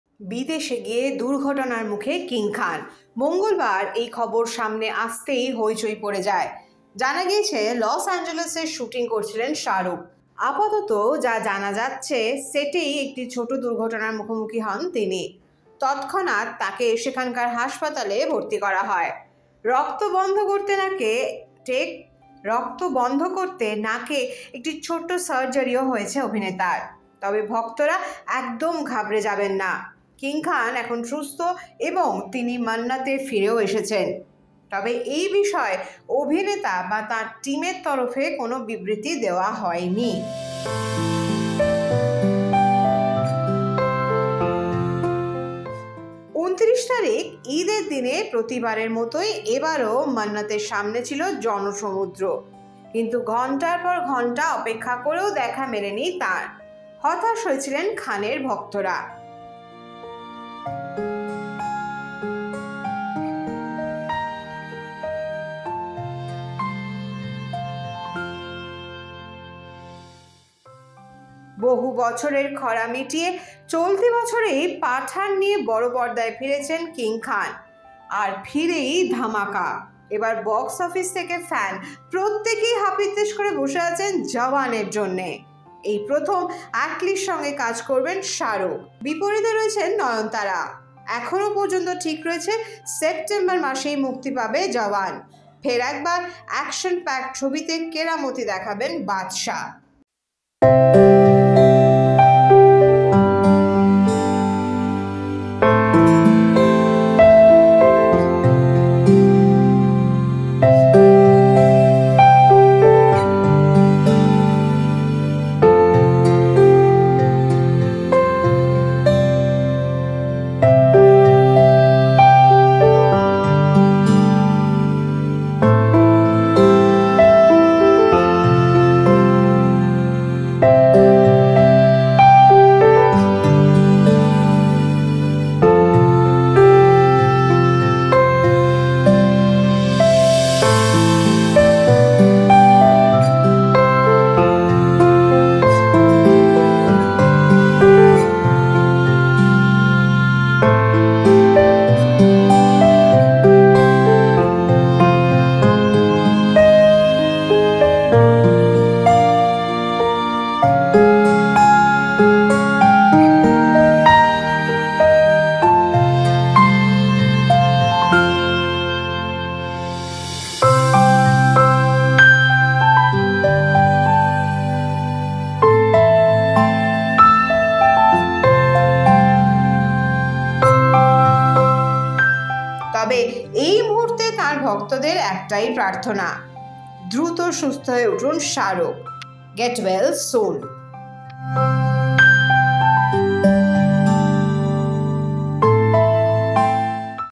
Shah Rukh Khan Injured : আমেরিকায় দুর্ঘটনার কবলে শাহরুখ! ফিরলেন দেশে | Entertainment News | Ei Samay
বিদেশে গিয়ে দুর্ঘটনার মুখে কিং খান (SRK)। মঙ্গলবার এই খবর সামনে আসতেই হইচই পড়ে যায়। জানা গিয়েছে লস অ্যাঞ্জেলসে শ্যুটিং করছিলেন শাহরুখ। আপাতত যা জানা গিয়েছে সেটেই একটি ছোট দুর্ঘটনার মুখোমুখি হন তিনি। তত্‌ক্ষণাত্‌ তাঁকে সেখানকার হাসপাতালে ভর্তি করা হয়। রক্ত বন্ধ করতে নাকে একটি ছোট্ট সার্জারিও হয়েছে অভিনেতার। তবে ভক্তরা একদম ঘাবড়ে যাবেন না। কিং খান এখন সুস্থ এবং তিনি মন্নতে ফিরেও এসেছেন। তবে এই বিষয়ে অভিনেতা বা তাঁর টিমের তরফে কোনও বিবৃতি দেওয়া হয়নি।